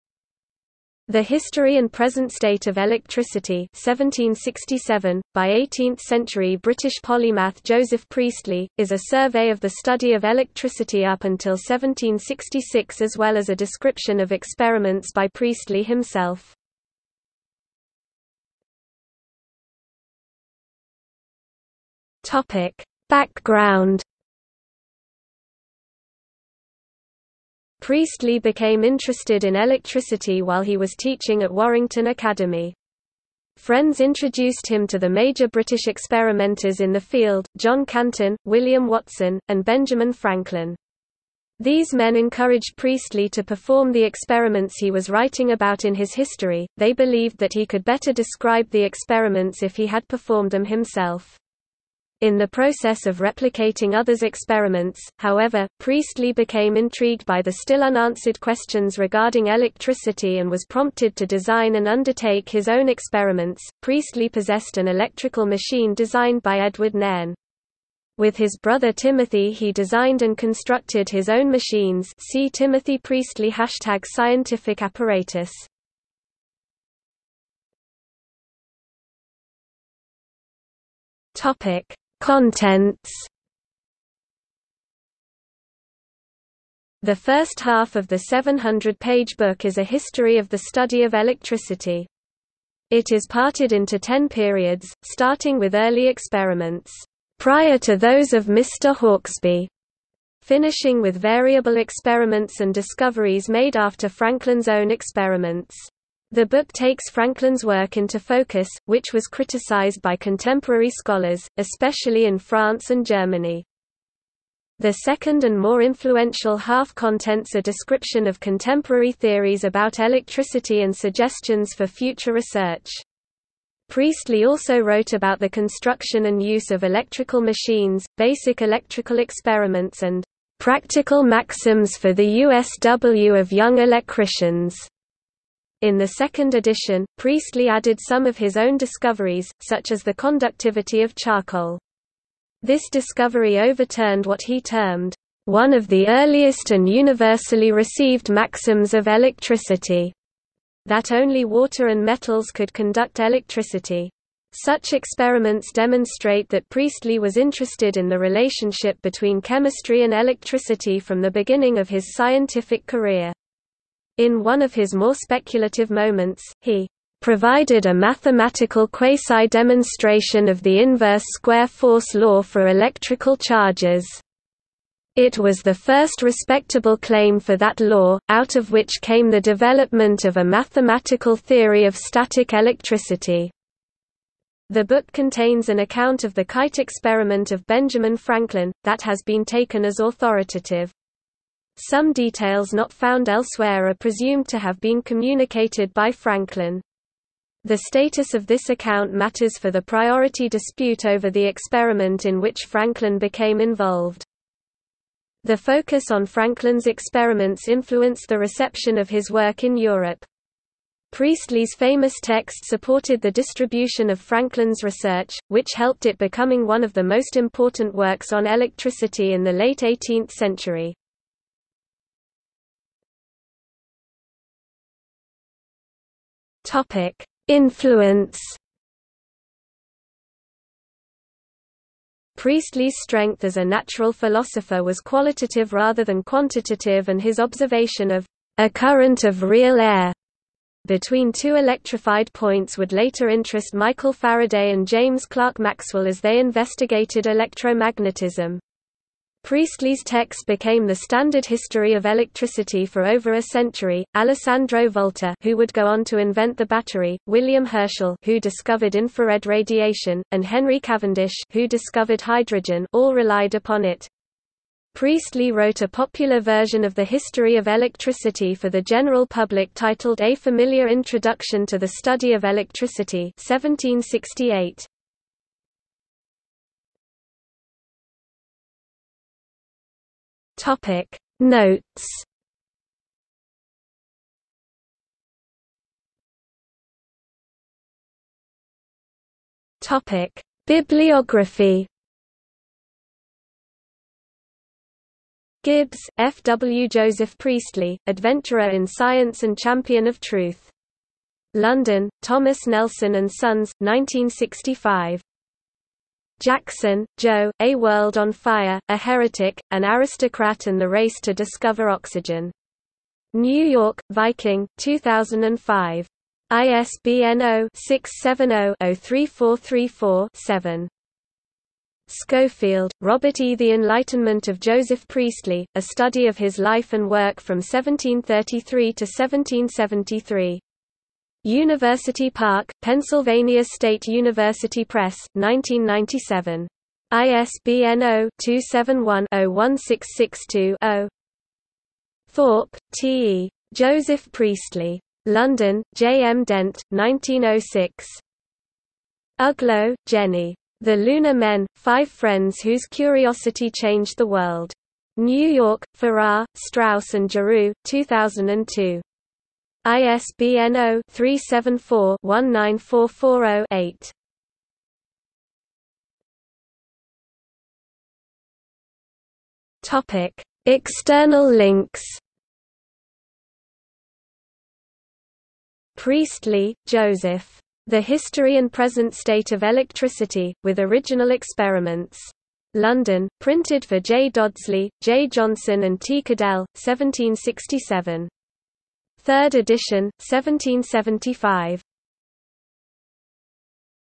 The History and Present State of Electricity | Wikipedia audio article
00:00:18 1 Background
00:01:17 2 Contents
00:03:43 3 Influence
00:04:36 4 Notes
00:04:46 5 Bibliography
00:06:06 6 External links

- Socrates

SUMMARY
The History and Present State of Electricity (1767), by eighteenth-century British polymath Joseph Priestley, is a survey of the study of electricity up until 1766 as well as a description of experiments by Priestley himself.